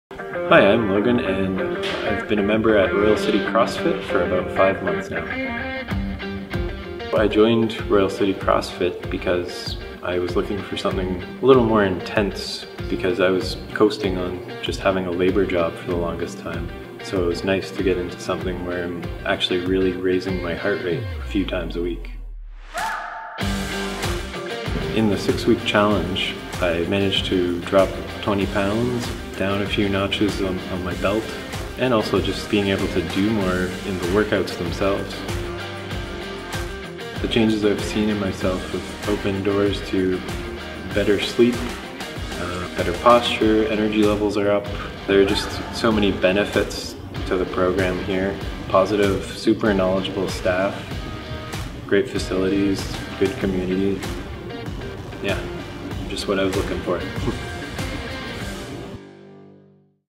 Logan's 6-Week Challenge Story
Here is Logan's story of getting started with Royal City CrossFit through our 6-week Transformation Challenge